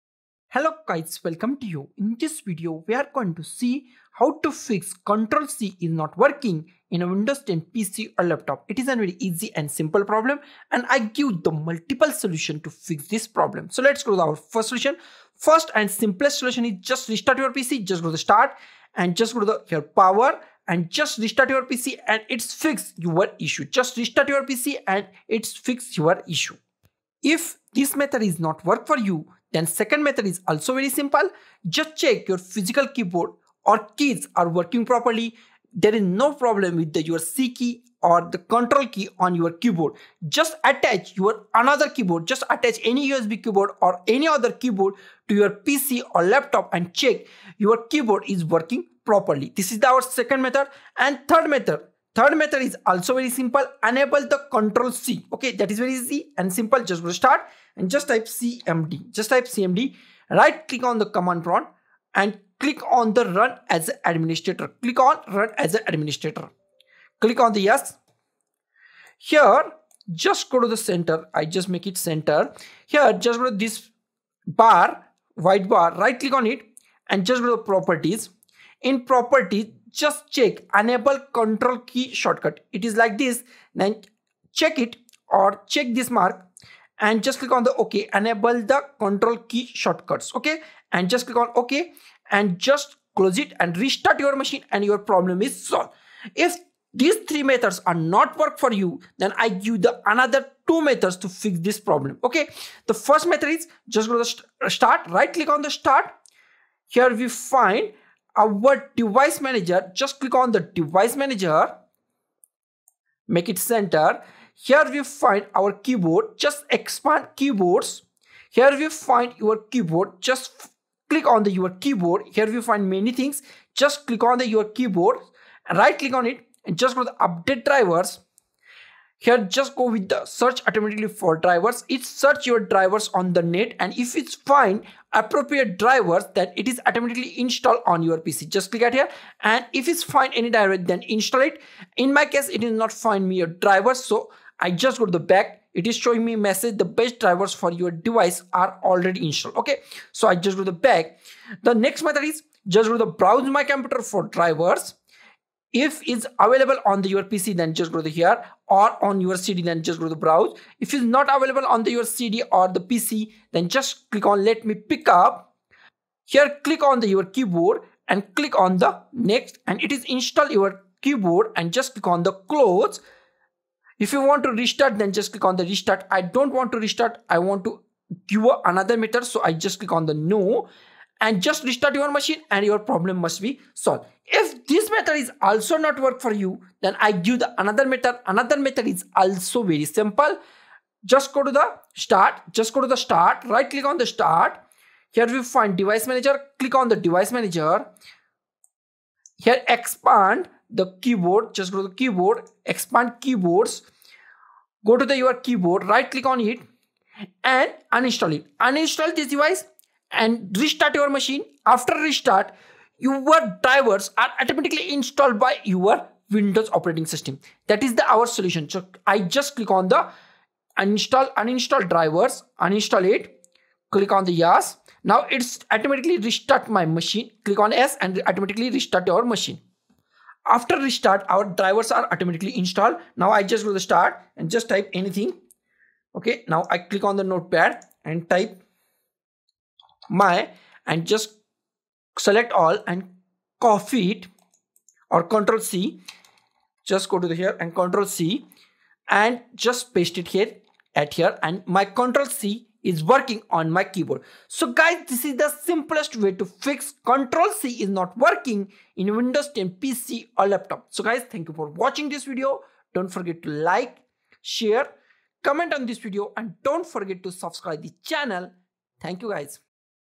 How to Fix Ctrl + C Not Working in Windows 10 PC or Laptop
Experiencing issues with your Ctrl + C keyboard shortcut not working on your Windows 10 PC or laptop? Don't worry, we've got you covered! In this comprehensive troubleshooting guide, we'll walk you through the step-by-step process to resolve this frustrating problem and get your copy-paste functionality back on track.

Whether you're unable to copy text, files, or any other content using Ctrl + C, this tutorial will address the common reasons behind this issue and provide practical solutions to fix it. We'll cover various troubleshooting methods, ensuring that you can effectively resolve the problem regardless of its underlying cause.

Our easy-to-follow instructions will guide you through Windows 10's settings, keyboard configuration, and other essential areas to identify and rectify any conflicts or glitches causing the Ctrl + C shortcut malfunction. You don't need to be a tech expert to follow these steps, as we've designed this tutorial to be accessible and user-friendly for all levels of computer users.

Don't let the frustration of a malfunctioning Ctrl + C shortcut hinder your productivity any longer. Join us in this guide and regain full control of your copy-paste functionality in Windows 10. Watch now and resolve the Ctrl + C issue on your PC or laptop today!

Timestamps:
00:00 - Introduction
00:15 - Restart Windows 10 to Fix Ctrl + C Not Working
00:26 - check your physical
keyboard is working
properly
00:50 - Enable Ctrl + C in Command Prompt
01:36 - Update Keyboard Drivers
03:04 - Uninstall & Reinstall Keyboard Drivers
04:14 - Conclusion